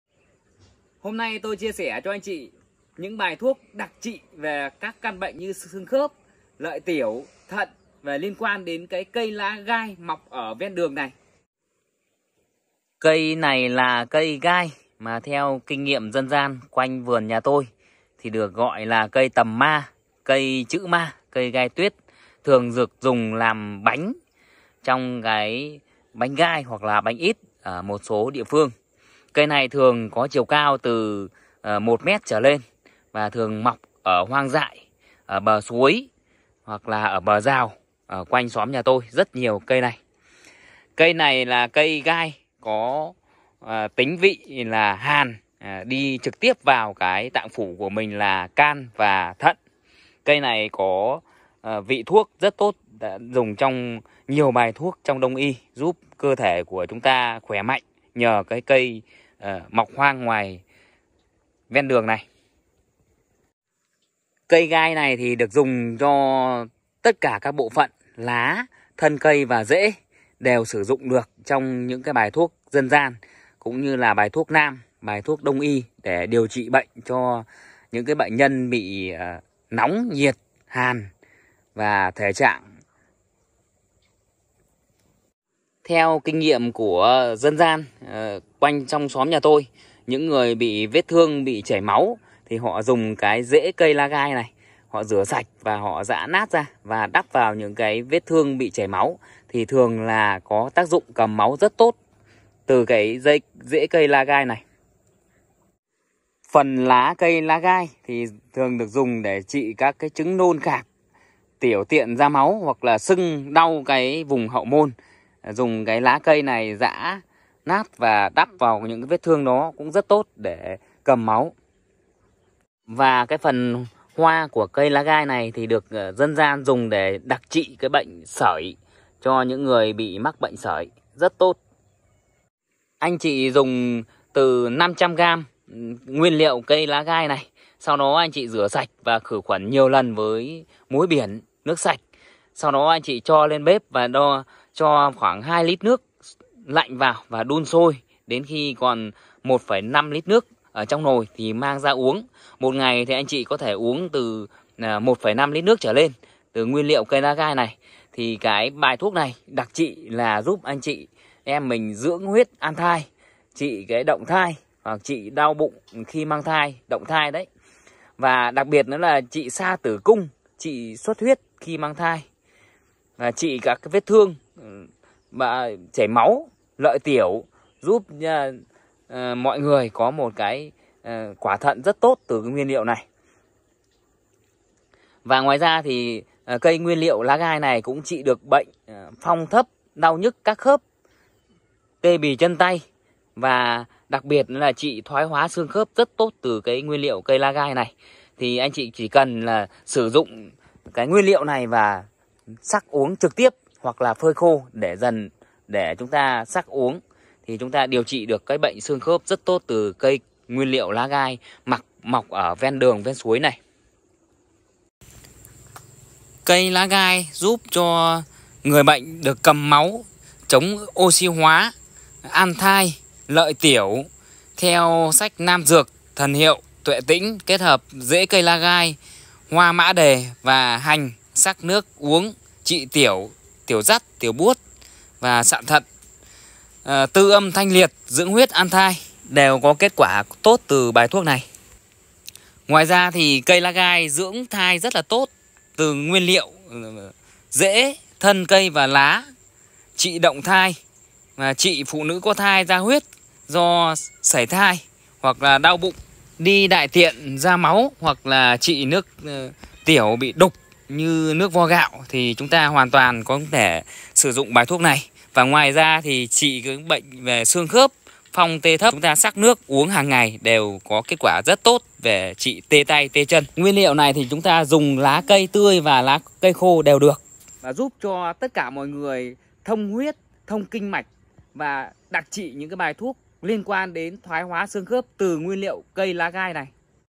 Bài thuốc dân gian từ cây lá gai
Cây lá gai còn được gọi là cây trữ ma, tầm ma, lá và rễ của cây không chỉ được sử dụng để làm bánh mà, còn được dùng để trị bệnh tiểu tiện đỏ, động thai, đau mỏi xương khớp và đại tiểu tiện ra máu. Ngoài ra, lá gai còn chứa nhiều thành phần dinh dưỡng và được nhân dân sử dụng như một loại rau ăn.
Bài thuốc nam dân gian tốt cho cả nam và nữ khi sử dụng nguyên liệu cây mâm xôi này để trị bệnh yếu sinh lý, khỏe mạnh cho nam nữ.
Cây gai không có độc nhưng có tính hàn, tránh sử dụng bài thuốc này cho người có thể trạng hư hàn hoặc sử dụng trong thời gian dài, ngoài ra để giảm thiểu rủi ro và tác dụng phụ khi điều trị.